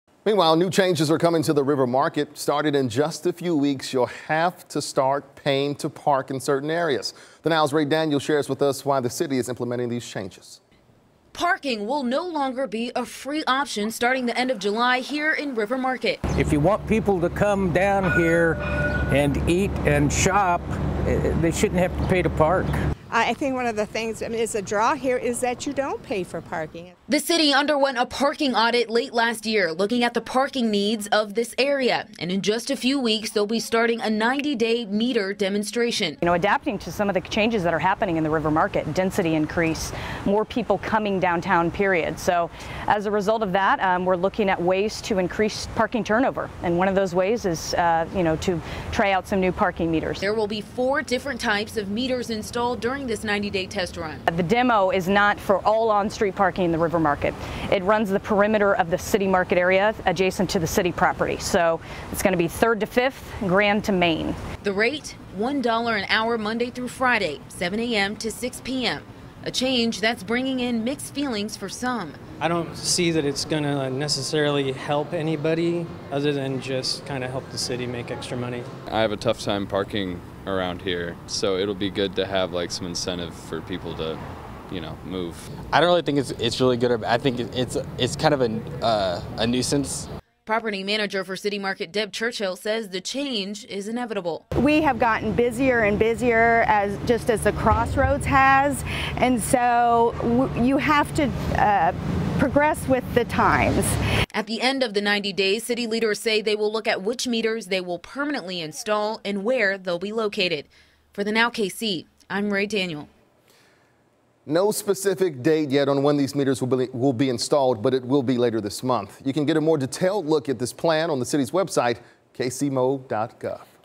River Market to test parking meters
As the River Market gets busier, the need for parking spaces has increased. The city has announced it plans to implement a 90-day testing period for parking meters on some streets that will be enforced Monday through Friday.